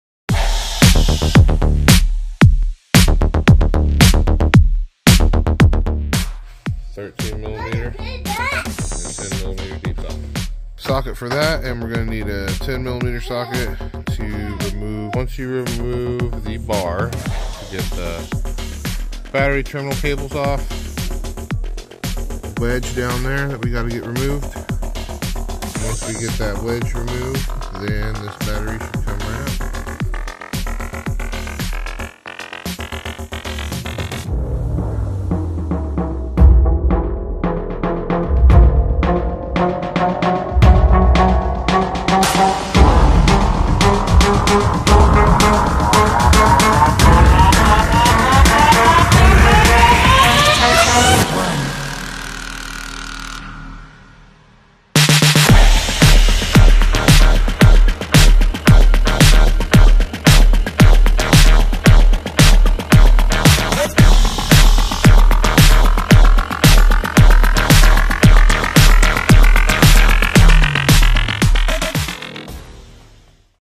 2018 Chevrolet Tahoe Battery Removal
2018 Chevrolet Tahoe Battery Removal. Chevy battery removal.
Video and tools list of removal process